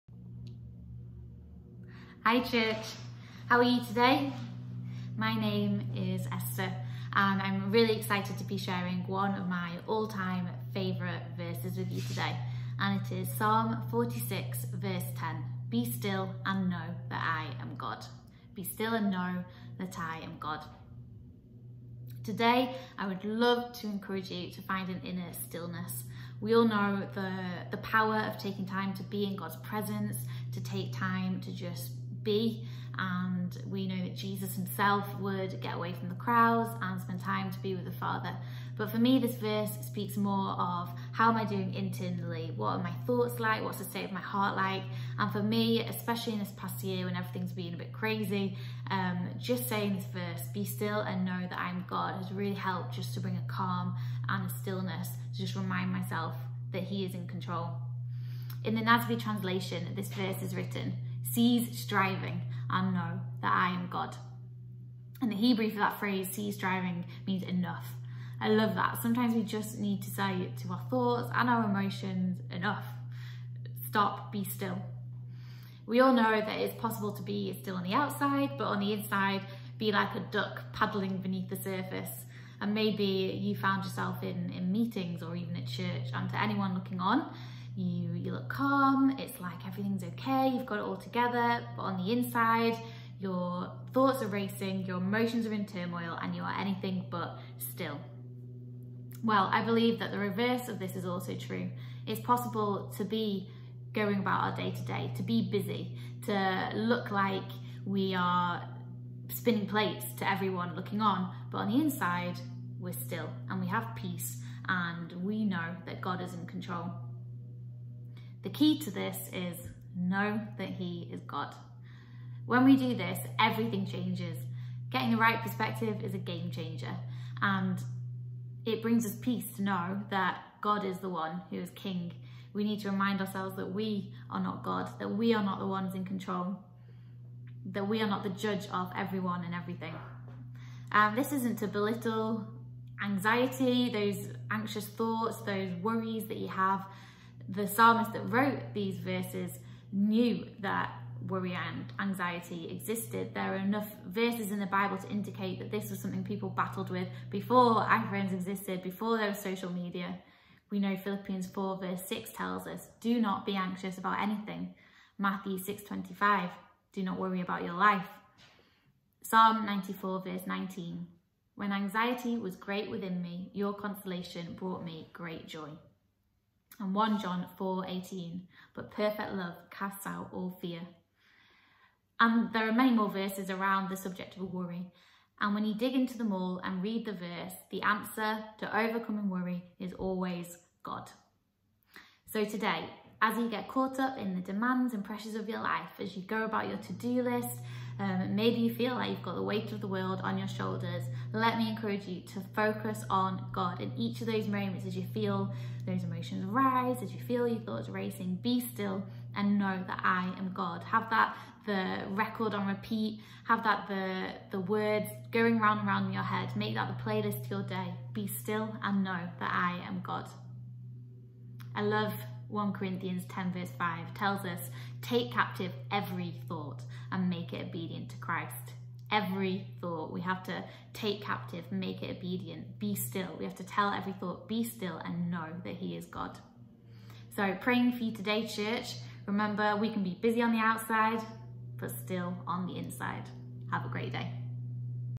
!Audacious Daily Devotionals - Friday 2nd July 2021
Today, Esther Dickenson part of our Chester location team, shares a Bible passage that has great meaning to her.